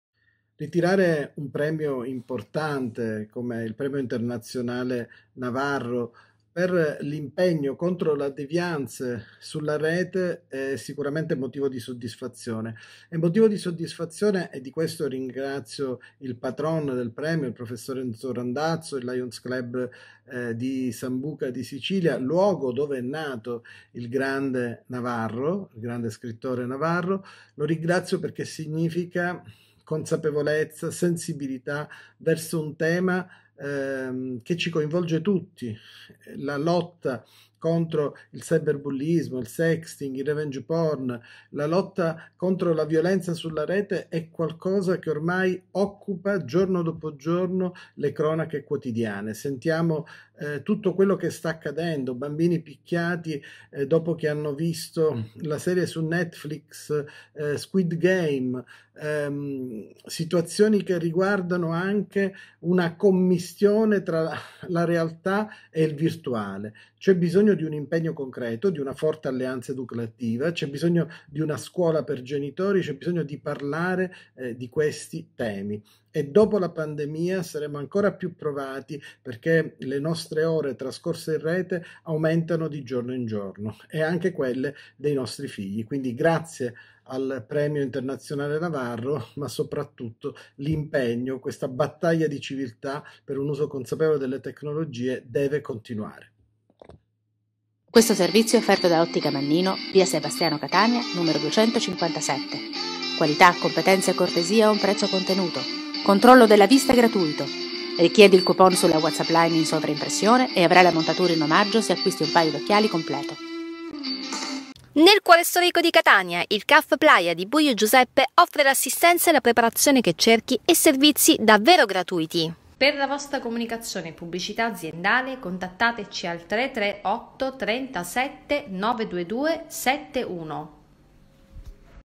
Premio Navarro 2021
L’evento, di cui è Patron il professor Enzo Randazzo, è stato organizzato dal Lions Club Sambuca di Sicilia nella cittadina dove è nato il grande scrittore. Il sociologo e saggista: “un riconoscimento che mi spinge a lavorare ancora di più e a non fermare nemmeno un attimo questa battaglia di civiltà per un uso consapevole delle nuove tecnologie”.